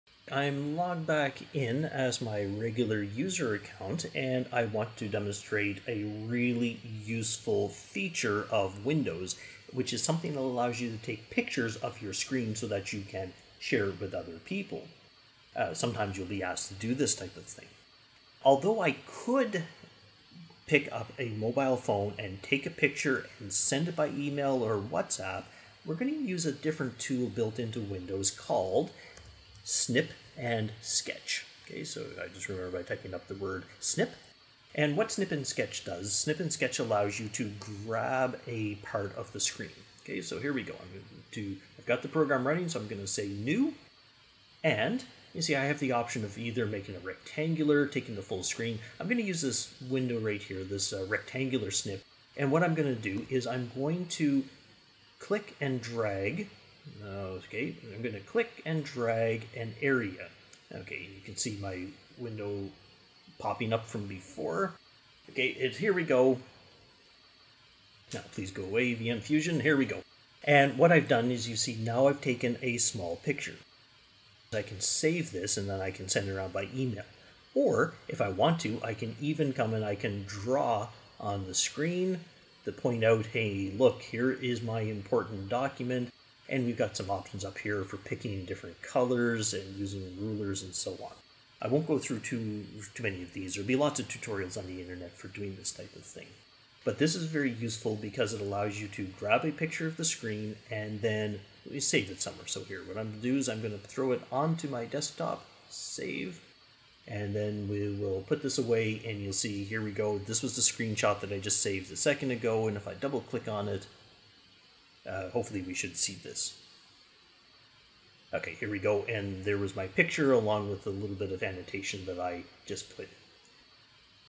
Windows 10: Snip and Sketch
How to grab parts of your screens and turn them into images.

This video is one of a set of small training videos for people just new to Windows 10 and/or Mac and need to learn how to perform basic tasks in these operating systems.